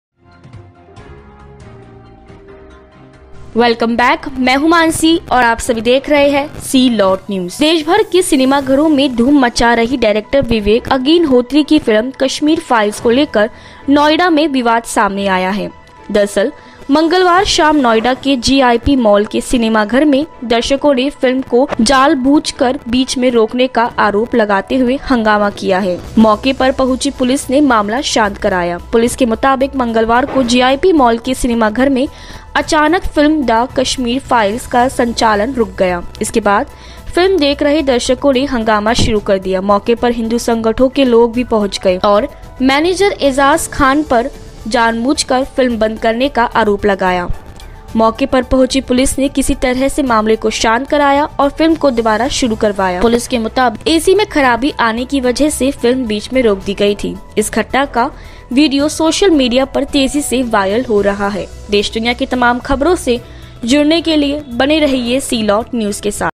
'The Kashmir Files' को लेकर Noida के GIP Mall पर हुआ बड़ा हंगामा , तेज़ी से viral हो रहा video
देशभर के सिनेमाघरों में धूम मचा रही डायरेक्टर विवेक अग्निहोत्री की फिल्म कश्मीर फाइल्स (kashmir files) को लेकर नोएडा में विवाद सामने आया है. मंगलवार शाम नोएडा के GIP मॉल के सिनेमाघर में दर्शकों ने फिल्म को जानबूझकर बीच में रोकने का आरोप लगाते हुए हंगामा किया है. मौके पर पहुंची पुलिस ने मामला शांत कराया
पुलिस के मुताबिक मंगलवार को GIP मॉल के सिनेमाघर में अचानक फिल्म 'द कश्मीर फाइल्स' का संचालन रुक गया. इसके बाद फिल्म देख रहे दर्शकों ने हंगामा शुरू कर दिया. मौके पर हिंदू संगठनों के लोग भी पहुंच गए और मैनेजर एजाज खान पर जानबूझकर फिल्म बंद करने का आरोप लगाया. मौके पर पहुंची पुलिस ने किसी तरह मामले को शांत कराया. और फिल्म को दोबारा शुरू करवाया. पुलिस के मुताबिक AC में खराबी आने की वजह से फिल्म बीच में रोक दी गई थी. इस घटना का वीडियो सोशल मीडिया पर तेजी से वायरल हो रहा है.